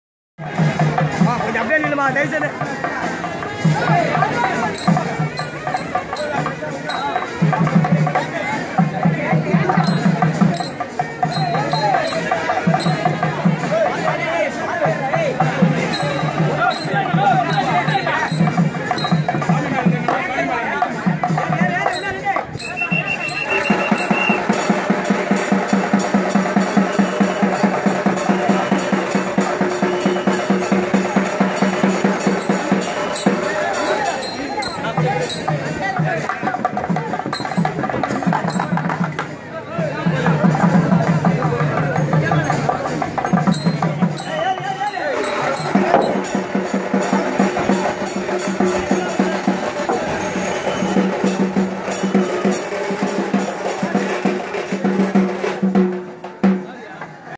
Karaneeswarar Ther 2017
Karaneeswarar Ther 2017 - the 'Ther thirumbudhal' at intersection of VS Mudali St and KP Koil St